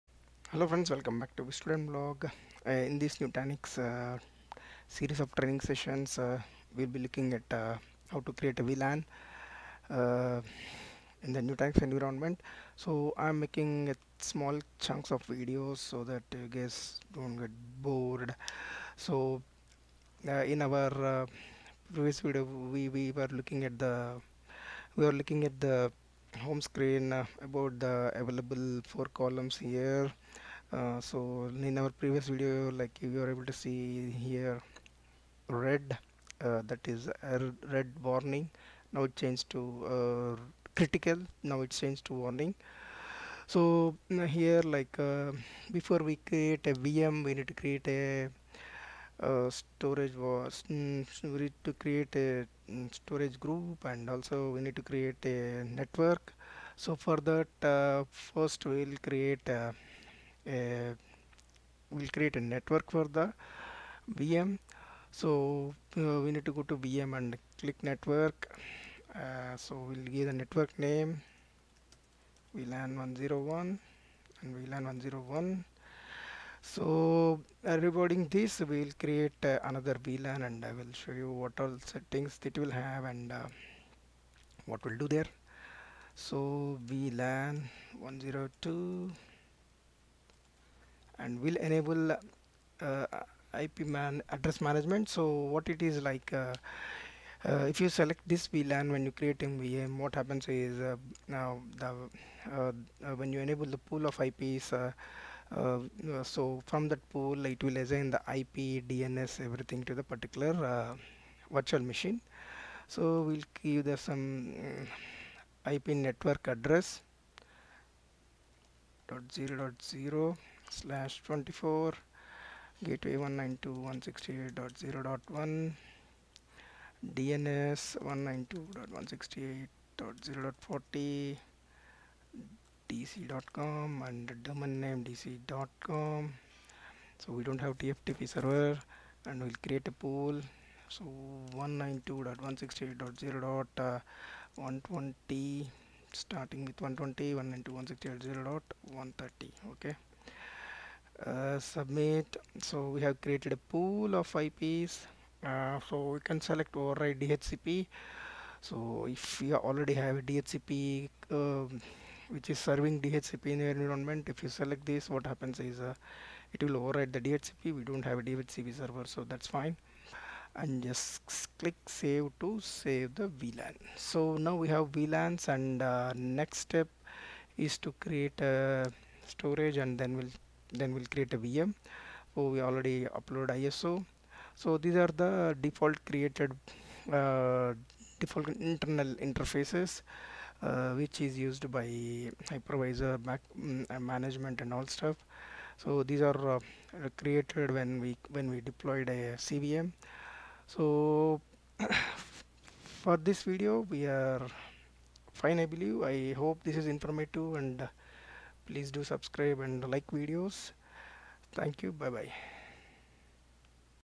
Nutanix - Creating vLAN - 07
In this Video it was shown how to create Vlans in Nutanix cluster